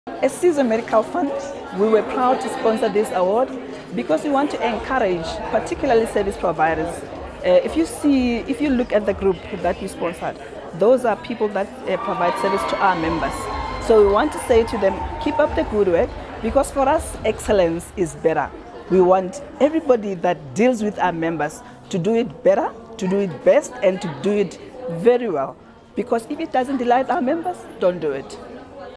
Neo Khauoe speaks about sponsoring the Top Women Awards 2015
Sizwe Medical Solutions were this year's sponsors of the category TOP GENDER EMPOWERED COMPANY.
Aspen Pharmacare Holdings was the winner of the 2015 HEALTH & PHARMA AWARD.
Top Women Awards 2015
Topco Media